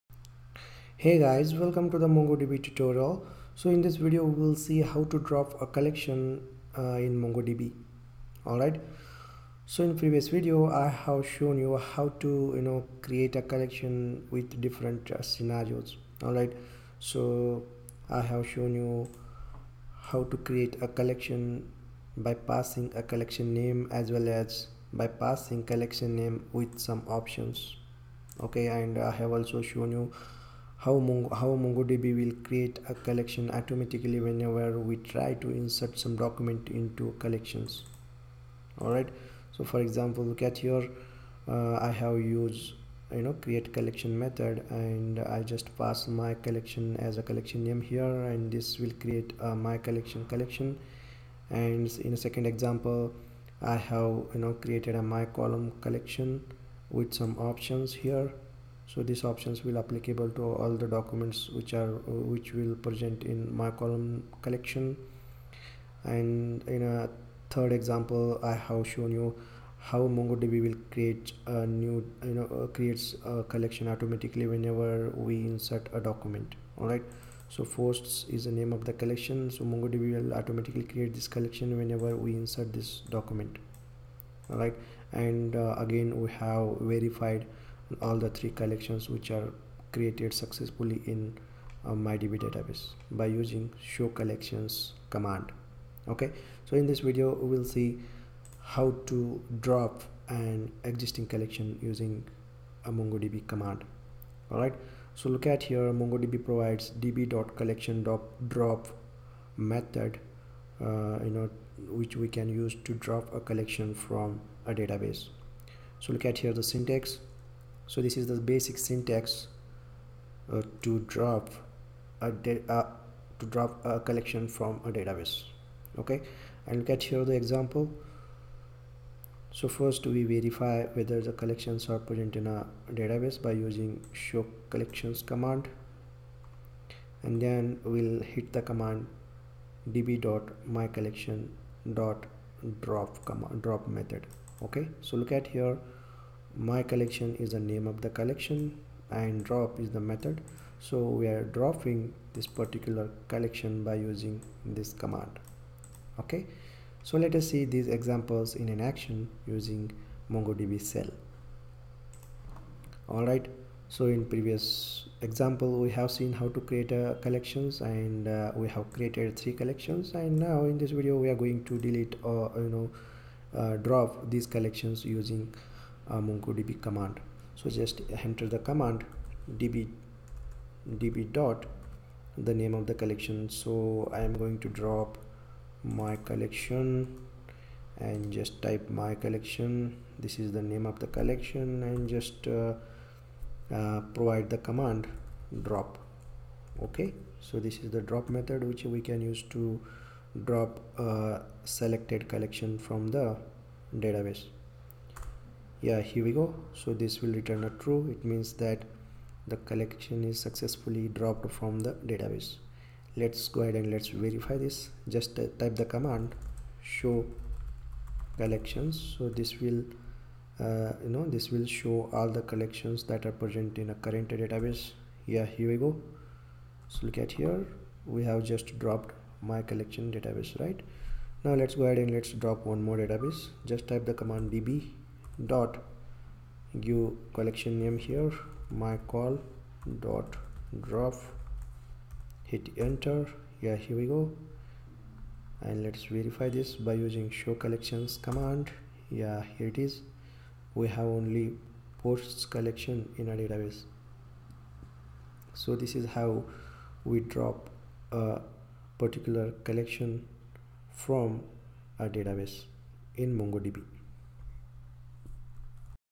MongoDB Tutorial 6 - MongoDB Drop Collection
This is part 6 of the MongoDB Tutorial.
In this video, we will see how to drop a collection from the database in MongoDB.